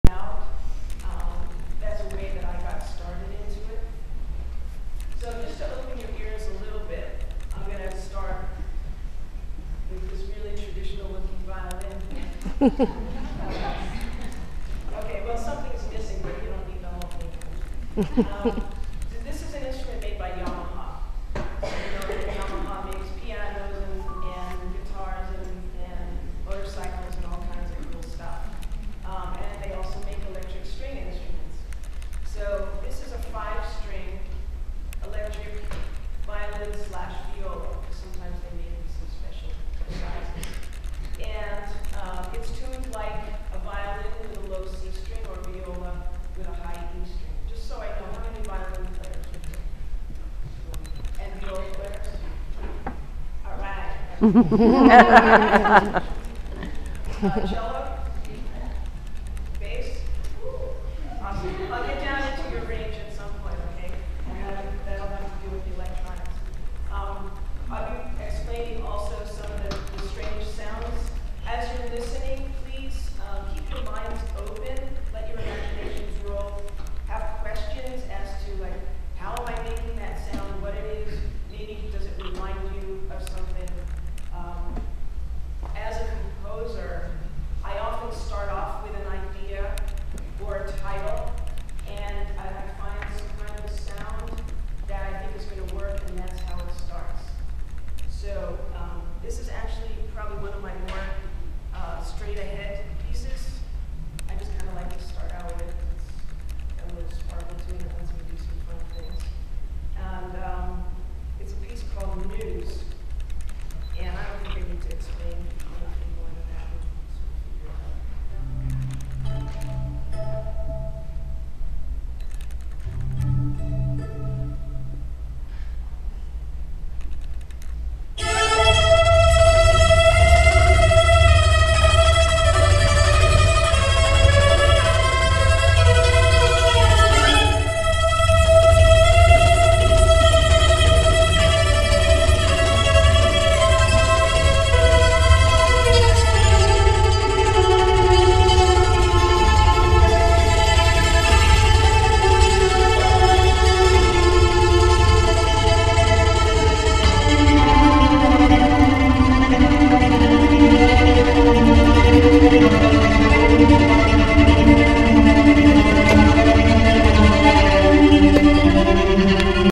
Plattsburgh.MP4
Martha Mooke presents a workshop on electric strings and improvisation to an engaged group of students in the Plattsburgh School District.  Sept. 2010